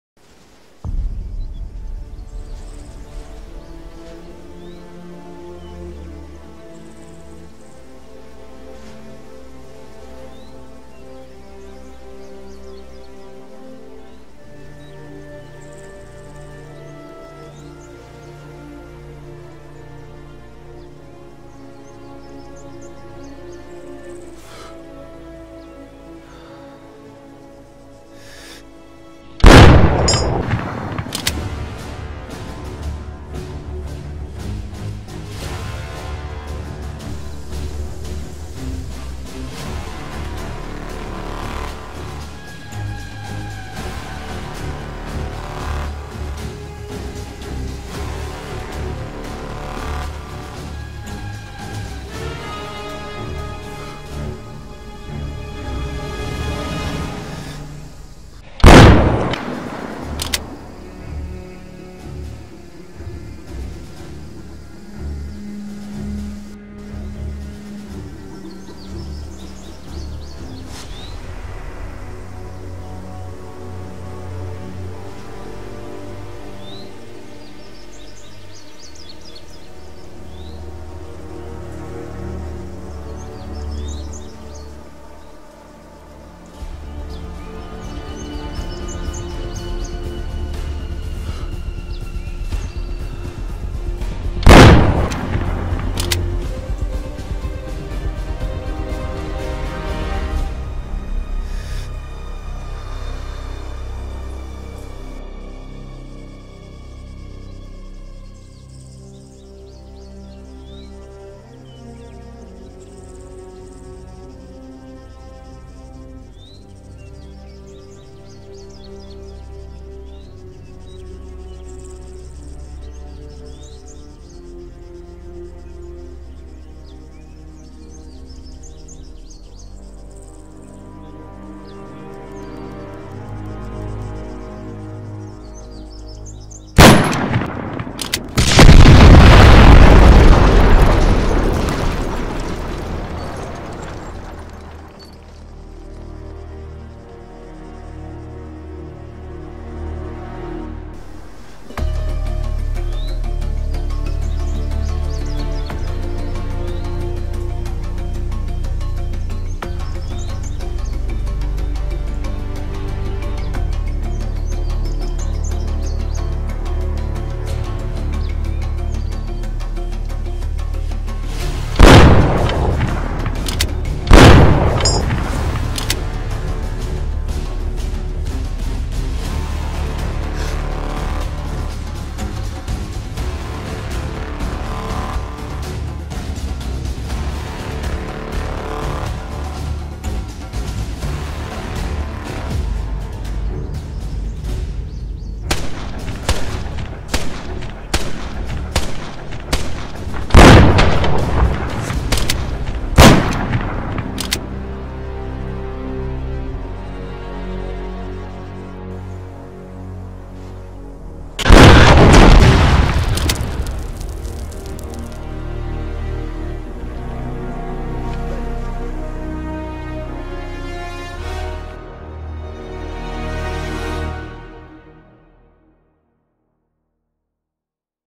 US Army snipers secure combat unit
Scharfschützen der US-Armee sichern Kampfeinheit